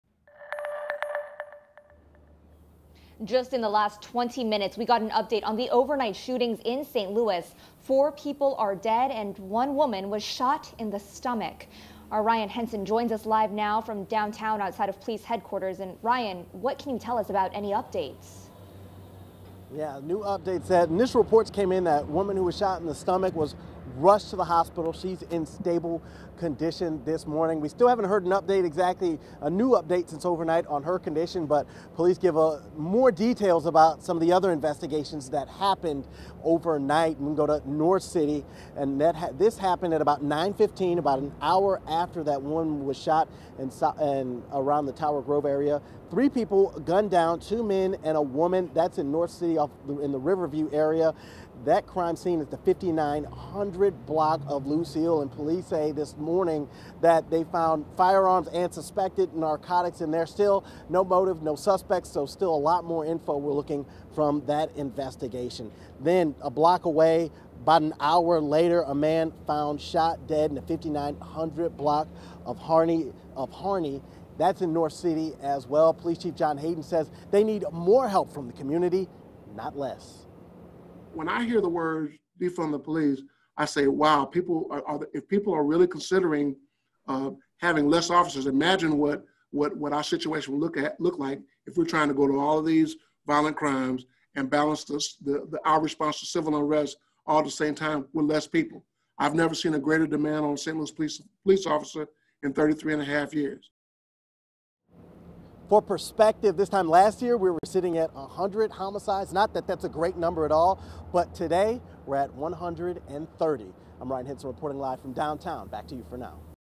4 killed, pregnant woman shot in stomach in 3 separate shootings in St. Louis
ST. LOUIS — Within the span two hours overnight, four people were killed and a pregnant woman was injured in three separate shootings across St. Louis.

One of the shootings was a triple homicide.

At 8:49 p.m., the St. Louis Metropolitan responded after a pregnant woman arrived at an area hospital with a gunshot wound to her stomach. She told police the shooting happened in the area of Manchester and Tower Grove avenues in the Forest Park Southeast neighborhood. That's also along a busy stretch of restaurants, bars and businesses in The Grove.

At 9:12 p.m., two men and a woman were found shot to death inside a home on the 5900 block of Lucille Avenue, which is near the border of the Walnut Park West and Walnut Park East neighborhoods. Police had no further information.

At 10:18 p.m., a man was shot in the head on the 5900 block of Harney a few blocks away from the triple shooting. He was pronounced dead at the scene.

There have been at least 129 homicides in the city this year.

This is a developing story. It will be updated as more information becomes available.